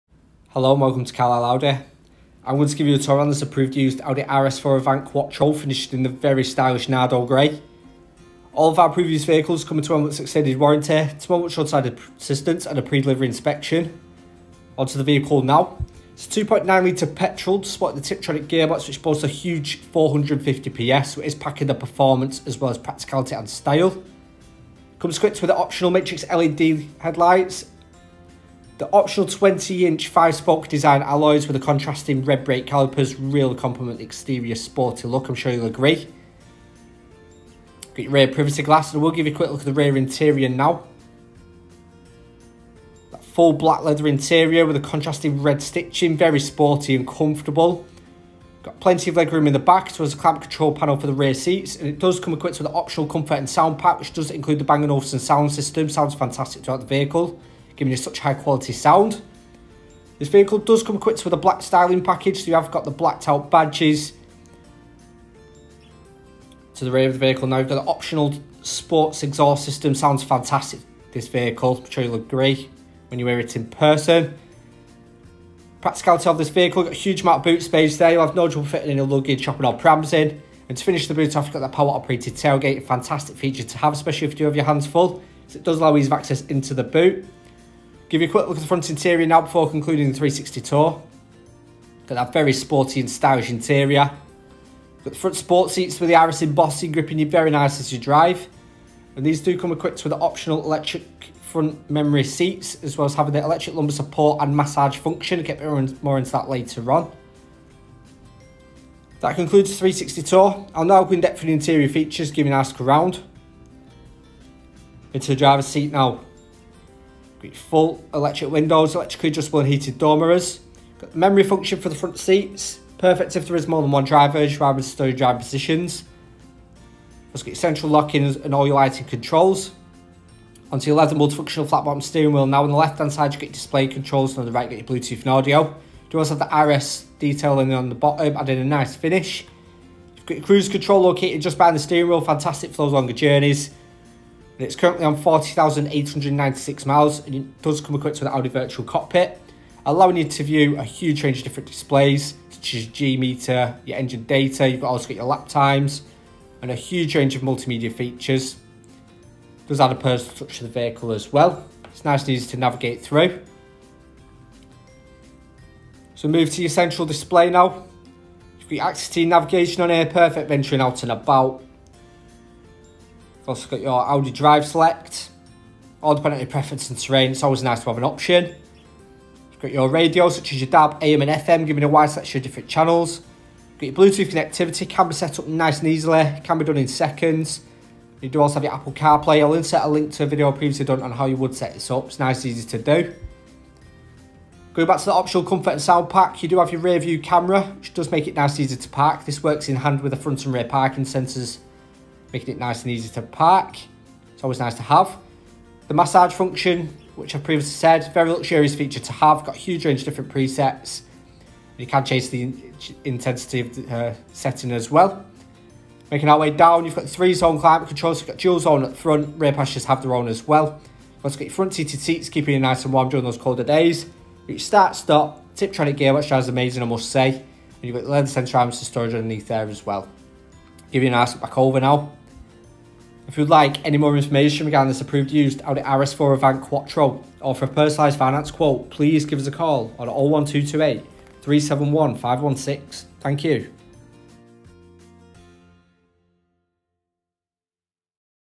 Approved Used Audi RS4 Avant Quattro | Carlisle Audi
Check out this Approved Used Audi RS4 Avant 2.9 TFSI quattro 450 PS tiptronic which is for sale at Carlisle Audi.

AP18KTT
40,896 miles
Petrol
Automatic

Features include:

Massage Seats
20" Alloys
Sports Exhaust System

Introduction: 0:00
Exterior: 0:15
Boot: 1:09
Interior: 1:30
How to get in touch: 3:53